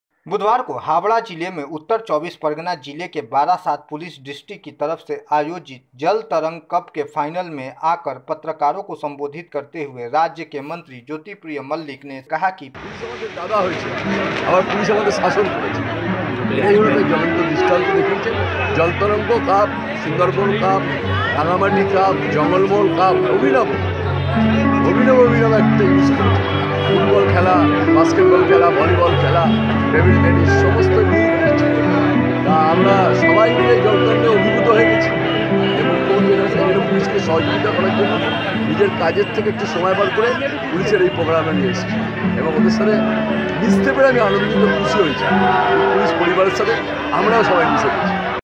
मंत्री ज्योतिप्रिय मल्लिक पहुंचे फूटबाल मैच के फाइनल में LIVE KOLKATA NEWS
13th december news.जो बंगाल के साथ देश भर की तमाम बड़ी ख़बरों को पेश करता है ।